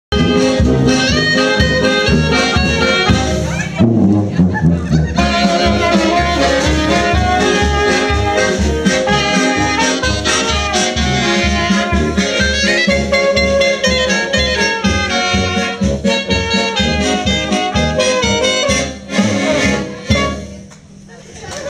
FootHills OomPah Band in Clayton NC
Oktoberfest in Clayton NC October 16-17, 2009